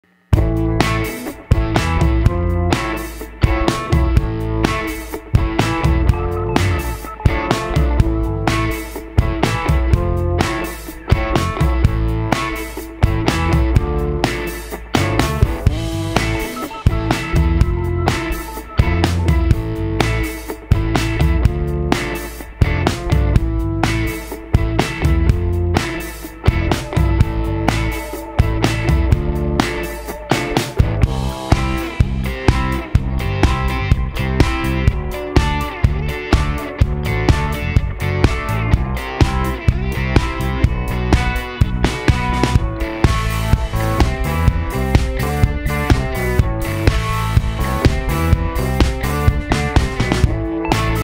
Hintertux meeting, Harald Harb and Reilly McGlashan Skiing Demo
Having fun skiing with Reilly.  Some short turns in Hintertux. He had the more difficult job to follow.